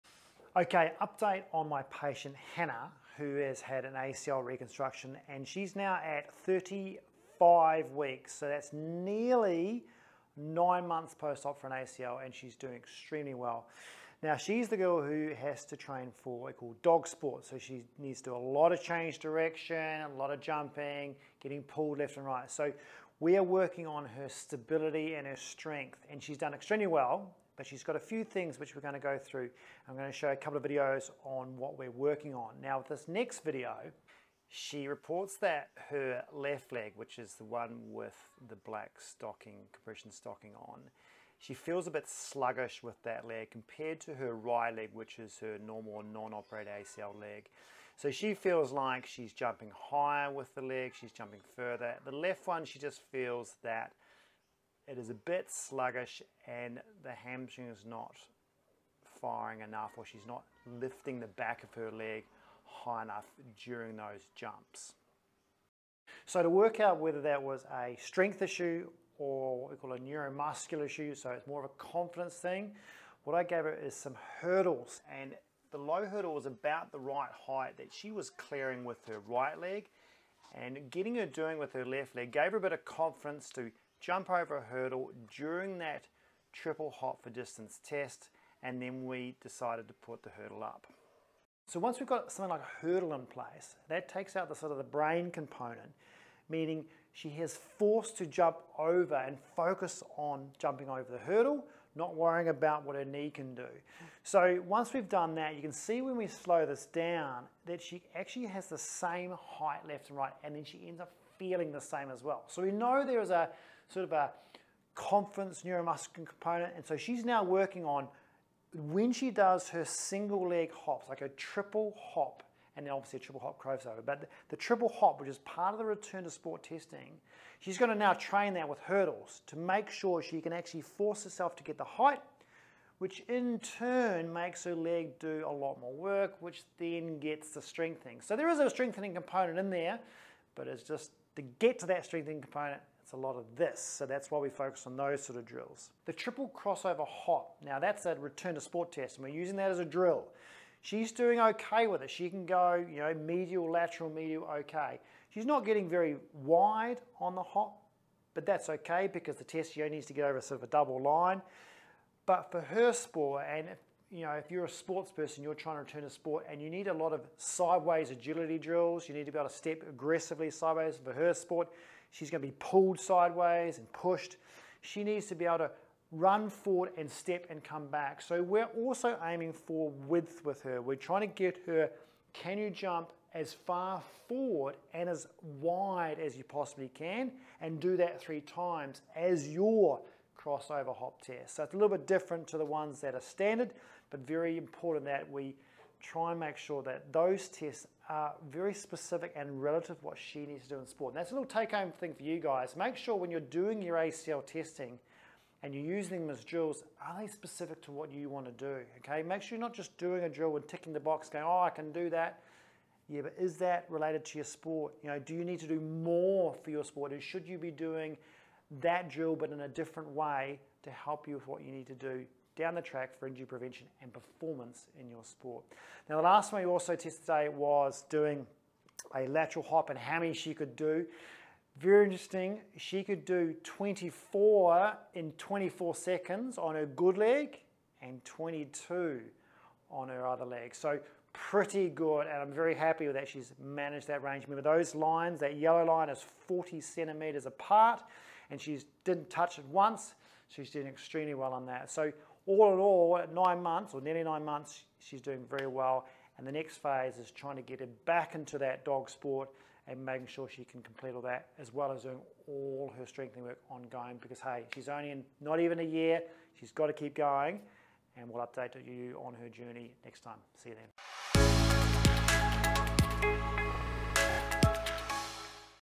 ACL Rehab RTS - Hopping Training at 35 weeks | Tim Keeley | Physio REHAB
ACL Rehab RTS - Hopping Training at 35 weeks

Back again with Henna who is at 35 weeks - nearly 9 months post-op. Doing really well and so we are working through using her Return to Sport (RTS) testing as DRILLS, to help her improve the hopping, jumping, lateral movement and side stepping for her Dog Sport that she is returning to.
To help her with the height of the hopping we have introduced hurdles in the triple hop for distance test.

1️⃣ Triple Hop for Distance
2️⃣ Triple Crossover Hop for Distance
3️⃣ Lateral Hop Test

Remember these are just SOME of the exercises she is doing each week. For the full ACL rehab program online with all the exercises, videos and protocols week-to-week, go

🌐 Telehealth Online Video Physio Consults